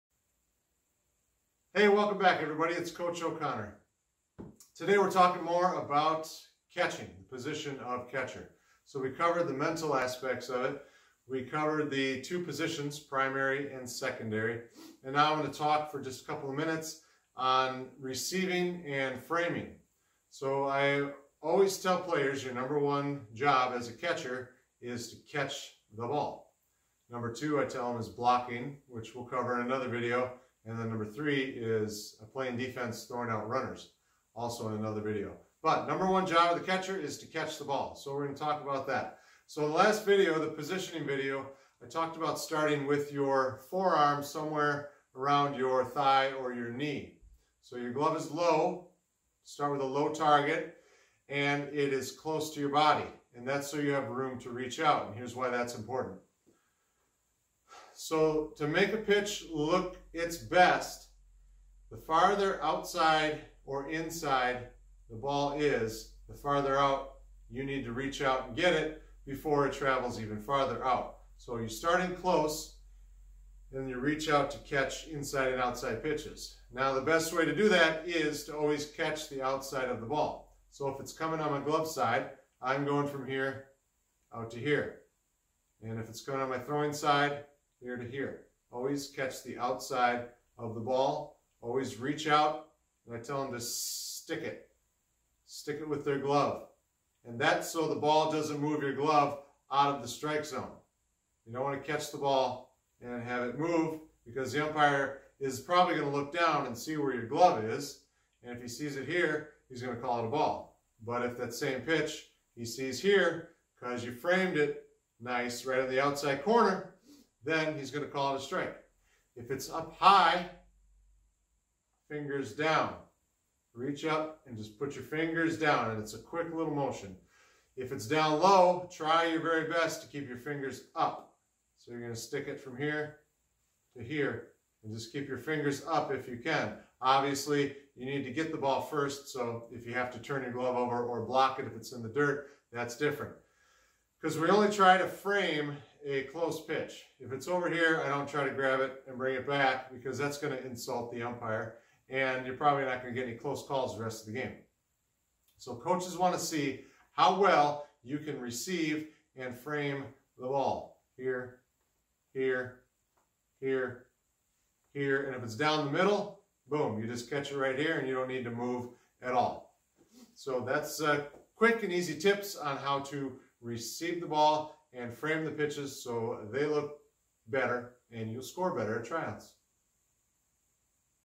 Catcher Receiving and Framing
The first job of the catcher is to catch the ball. Here are a few tips to help you receive the ball and to frame those close pitches so they look a little better!